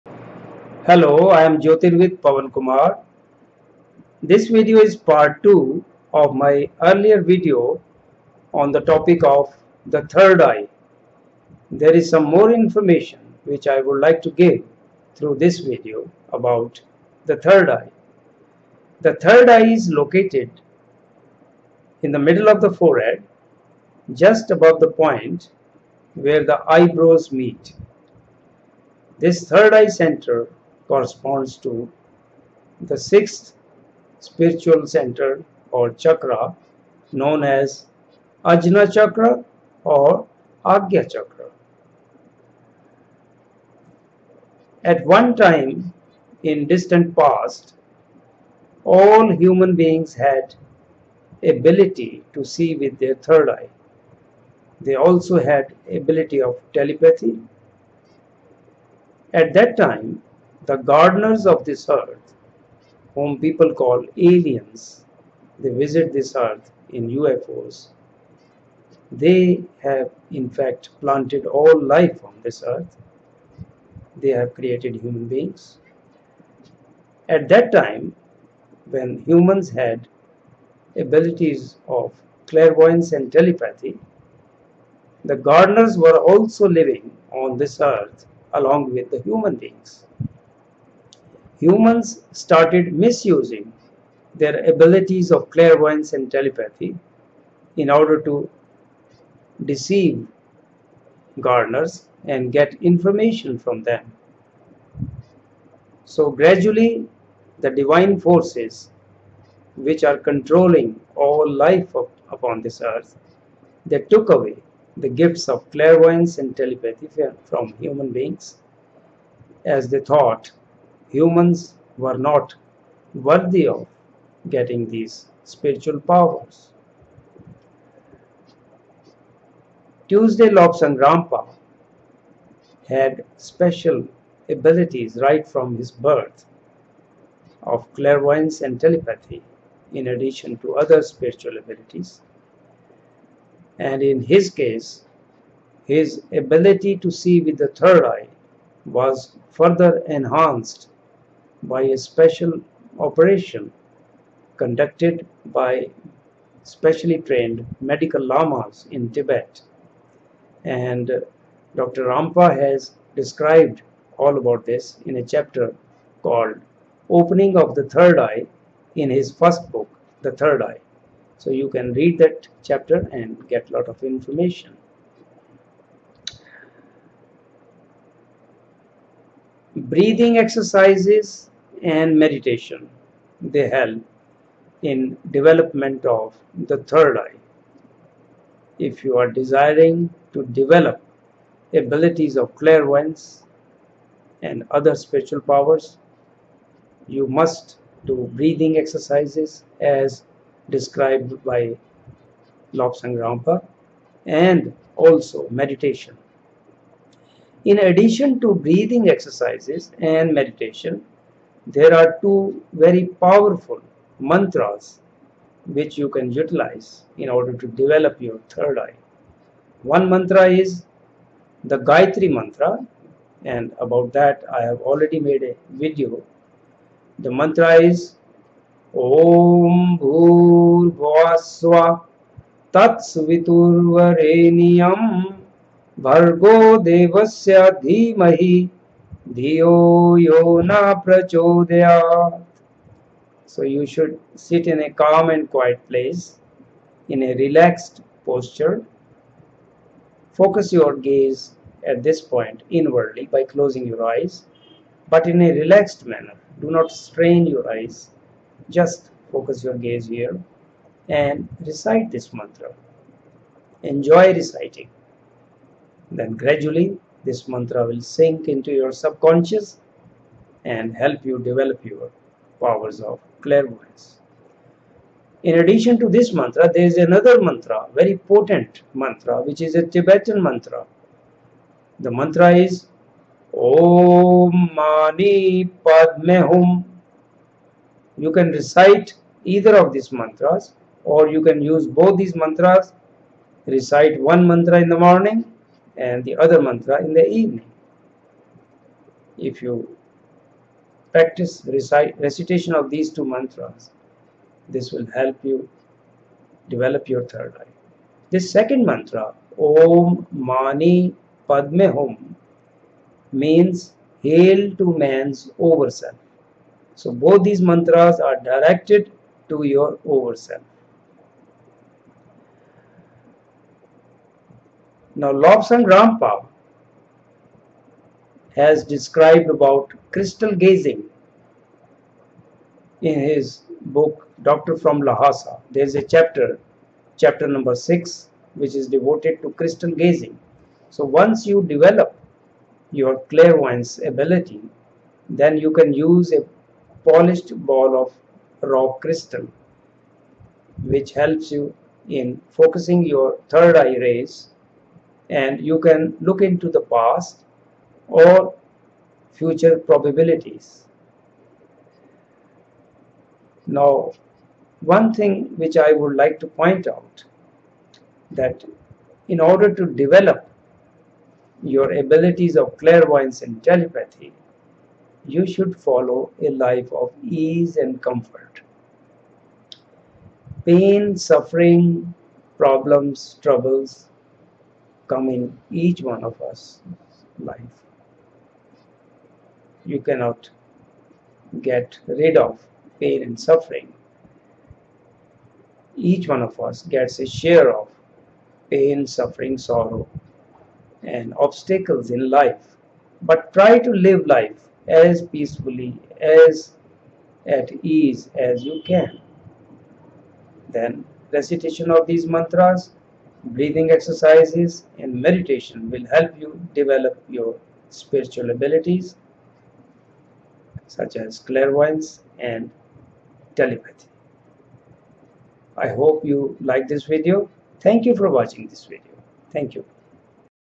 The Third Eye : Video Part-2
This Part-2 video of my earlier Part-1 Video on the third eye gives some more useful information on the topic.